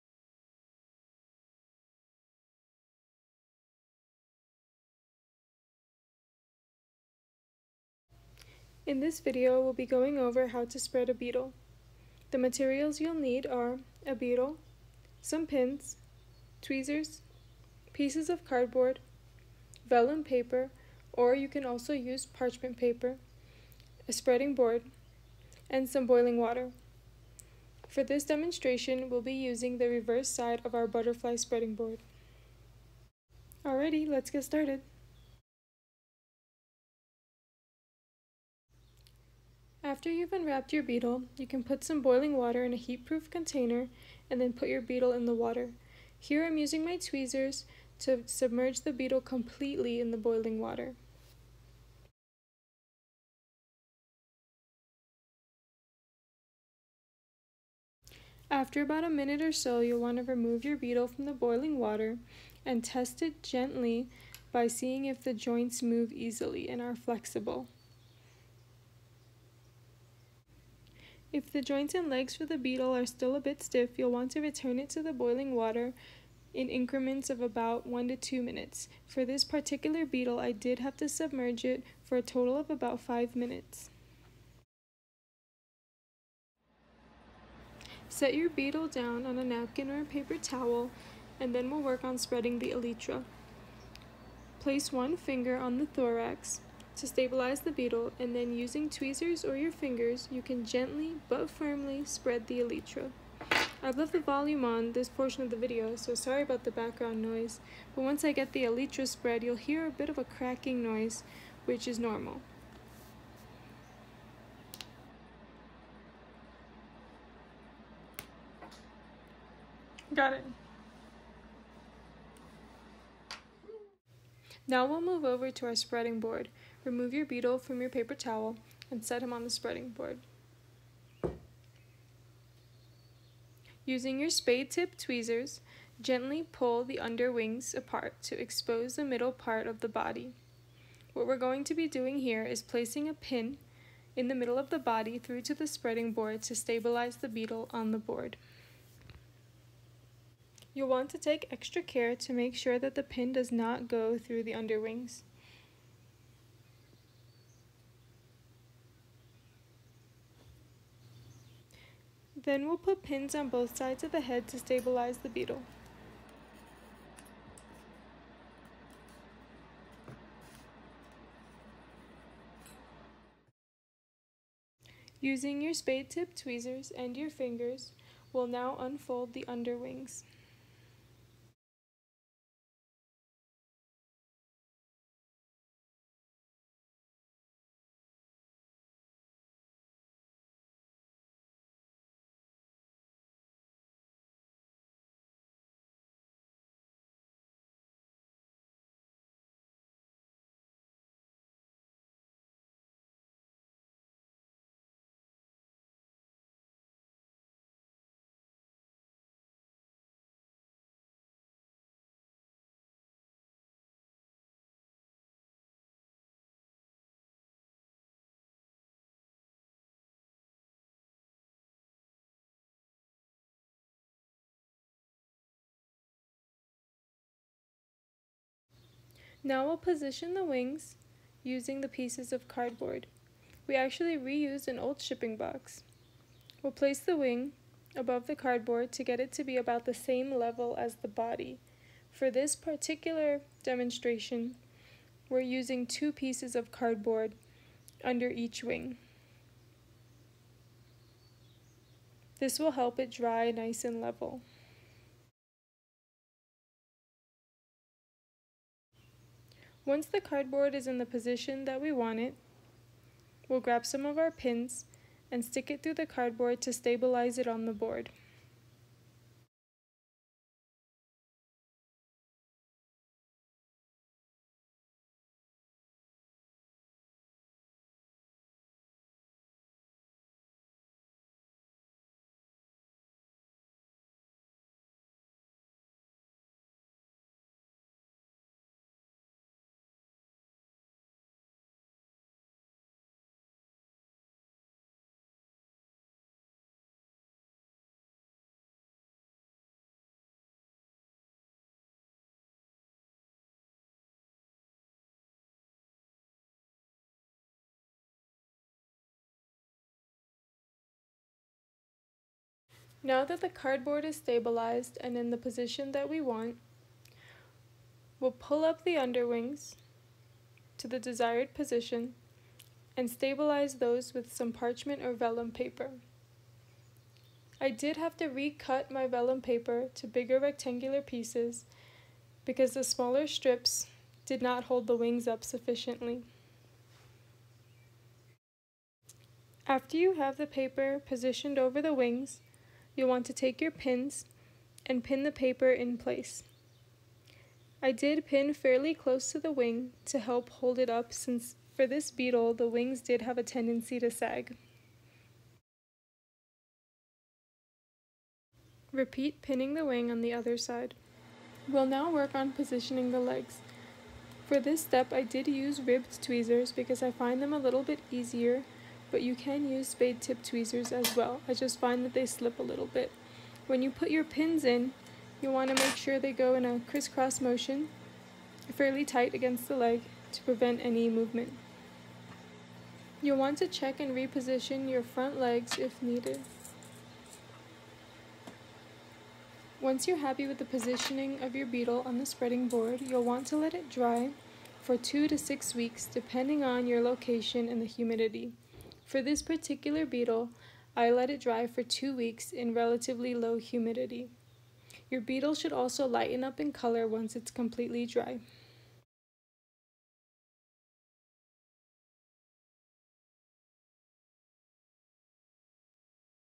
How to Spread a Beetle
￼ Instructions on how to pin and spread a beetle, including how to open and mount their wings.  More at the